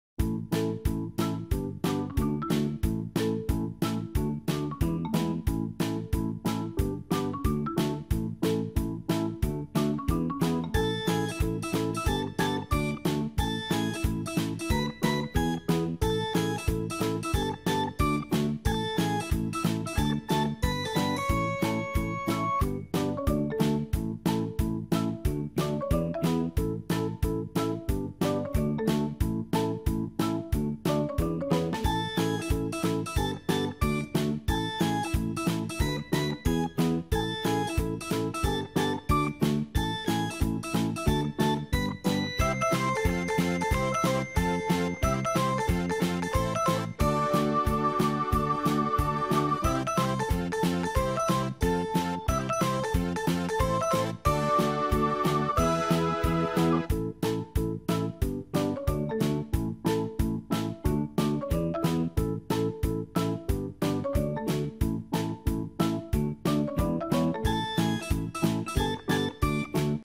Unboxing TOYS Review/Demos - Tomica 5 antique classic retro sports cars
Grandma toystore brings to you the unboxing of the Tomica 5 antique classic retro sports cars